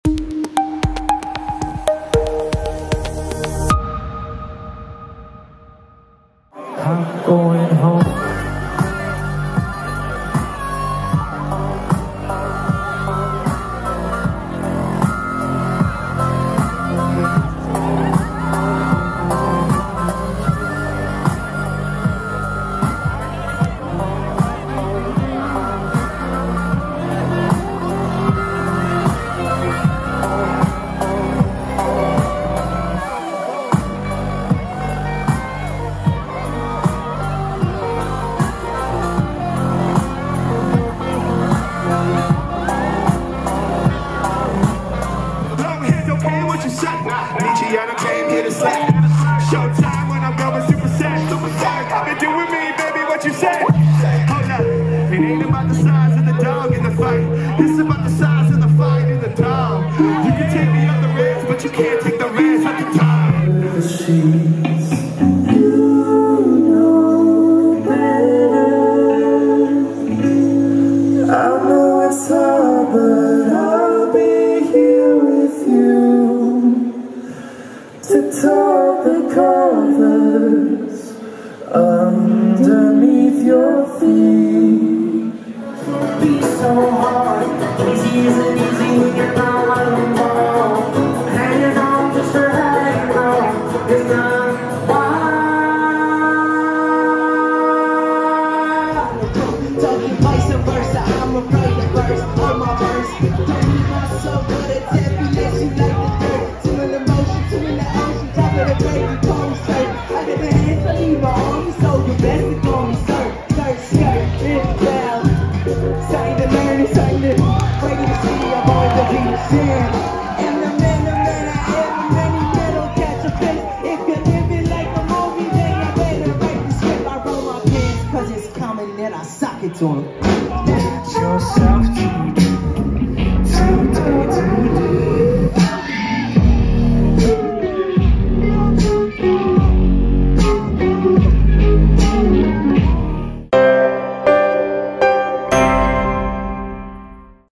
Fireside Fest highlights
Fireside Festival once again took over downtown Kelowna this past weekend, with some warm tunes for the winter doldrums.